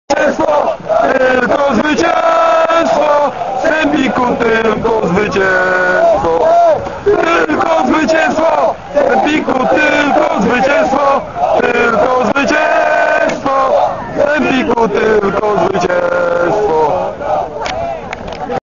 2005.10.16 Sęp Żelechów - Start Otwock: śpiew kibiców
Kibice wspierający swoim dopingiem piłkarzy Sępa w meczu ze Startem Otwock.